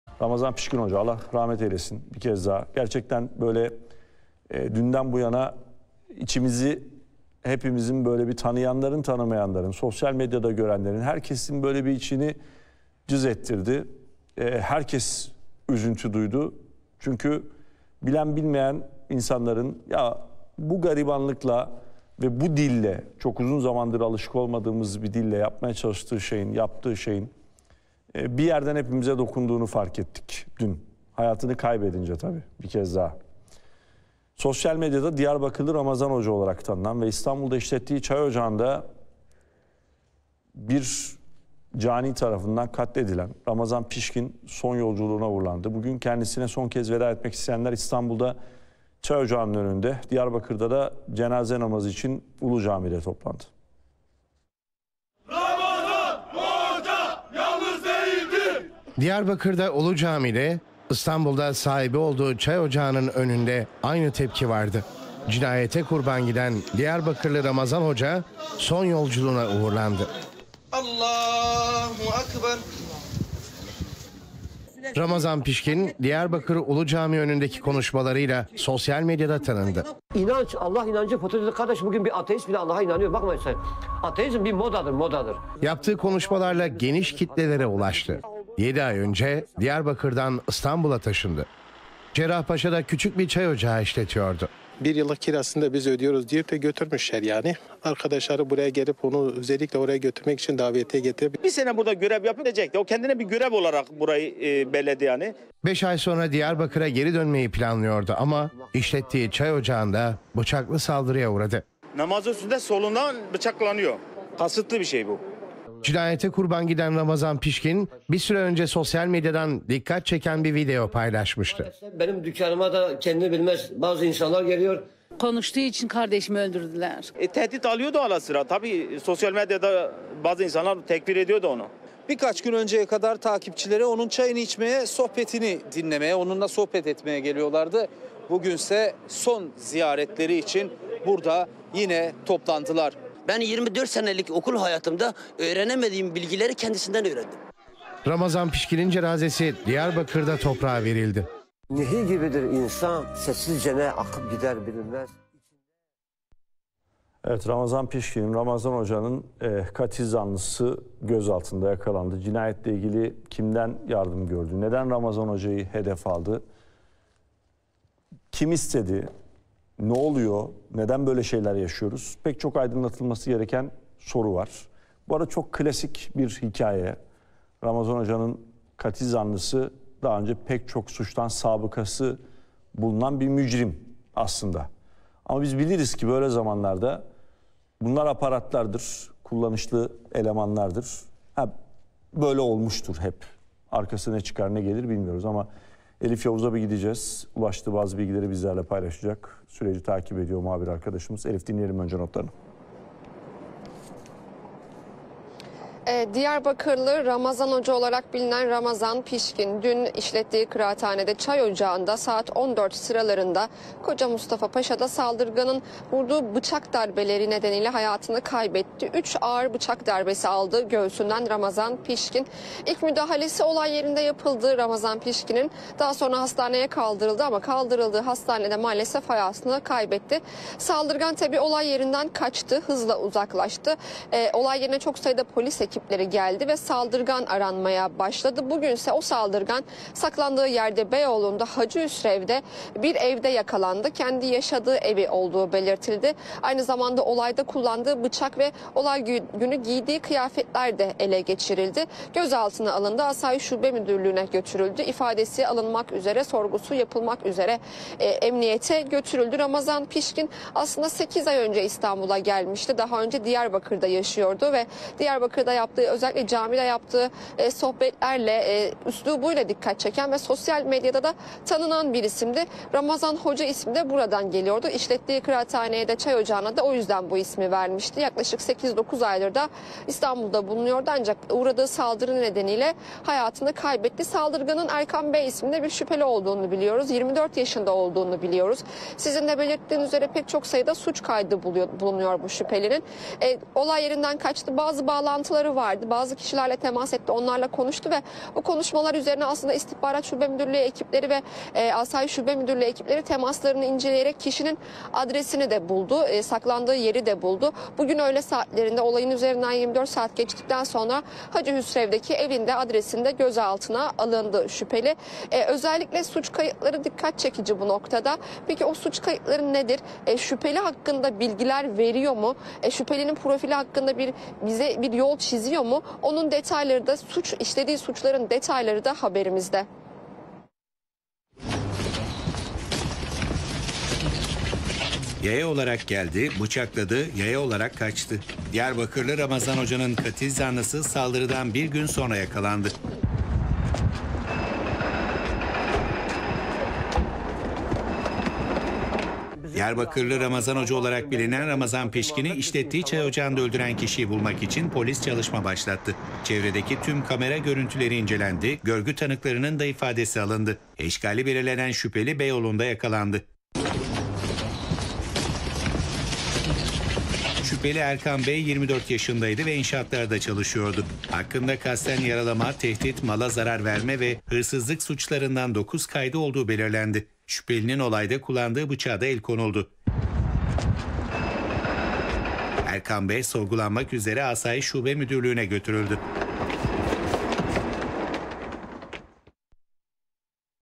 Neden Ramazan Hoca'yı hedef aldı? Katil zanlısı ilk ifadesinde ne dedi?
Diyarbakırlı Ramazan Hoca kimdi? Neden Ramazan Hoca'yı hedef aldı? Katil zanlısı ilk ifadesinde ne dedi? Ramazan Hoca kimi rahatsız etti?

Habertürk TV Muhabiri Elif Yavuz aktardı.